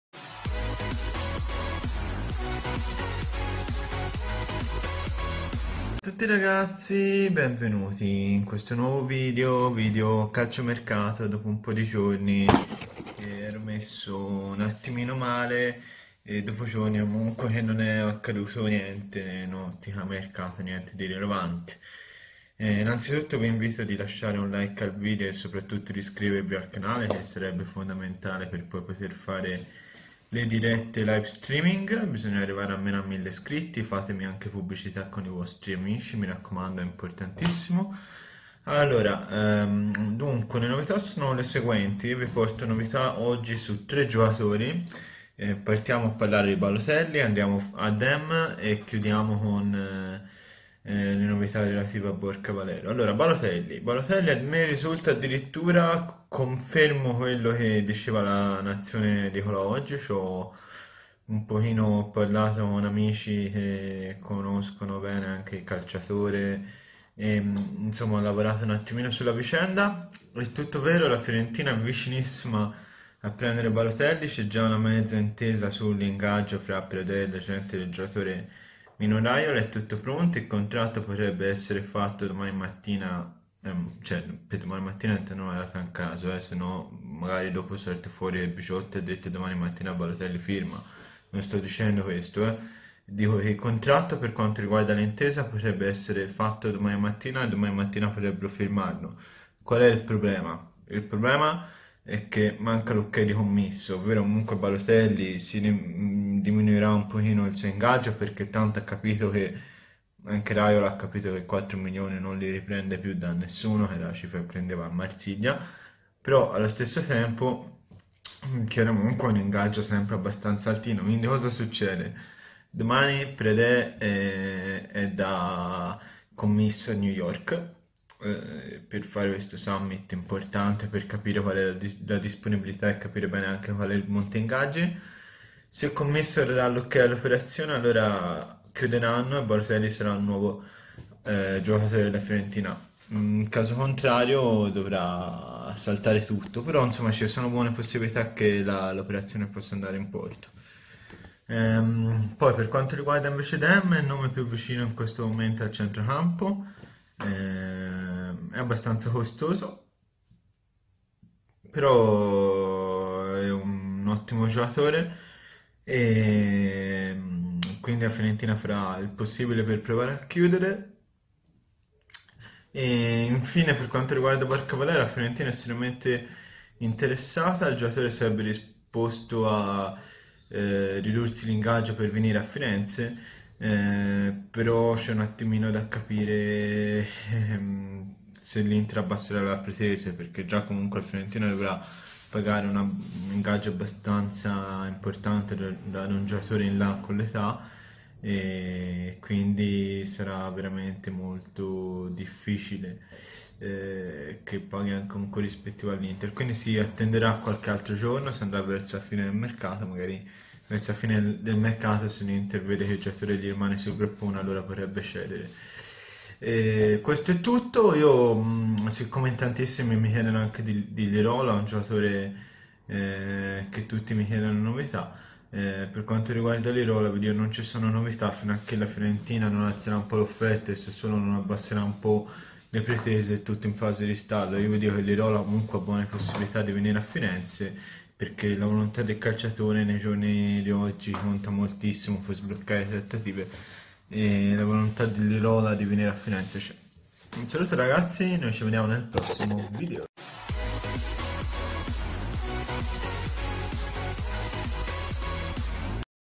BALOTELLI VICINISSIMO, CONTRATTO PRONTO. BORJA VALERO PUO' VENIRE, MA... PUNTO SU DEMME E LIROLA
Tutte le novità sul mercato viola
Contatti:
FB Lorenzo Bigiotti
YT Ibigio 96